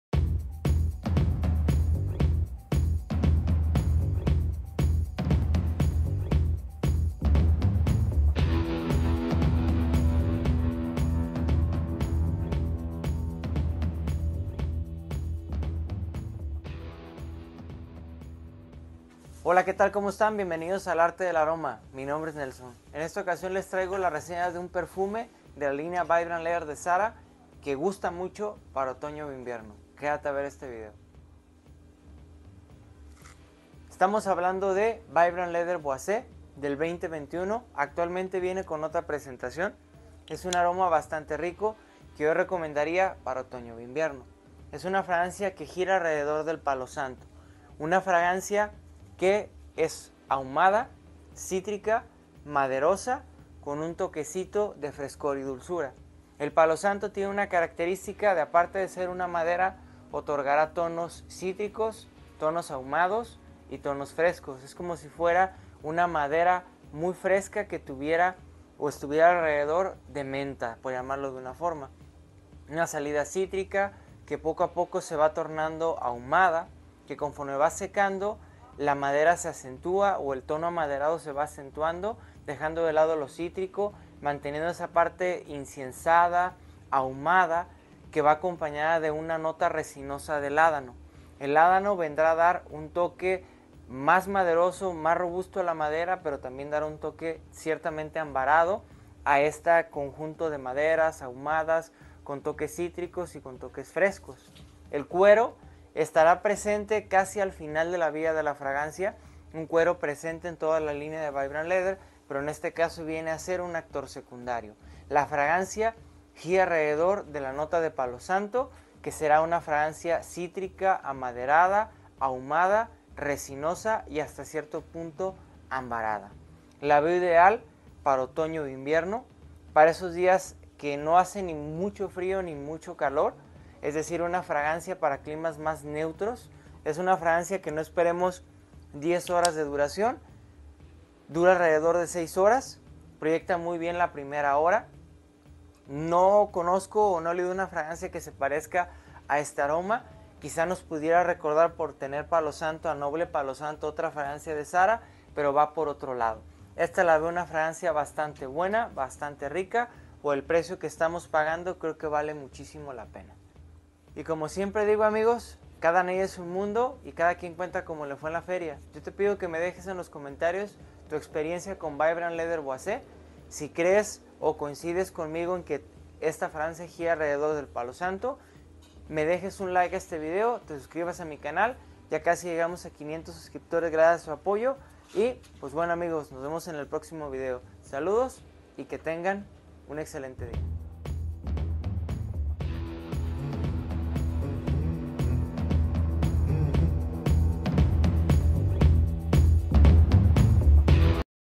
Vibrant Leather Boise de Zara
El día de hoy hablaremos de Vibrant Leather Boise de Zara, ¿es un buen flanker?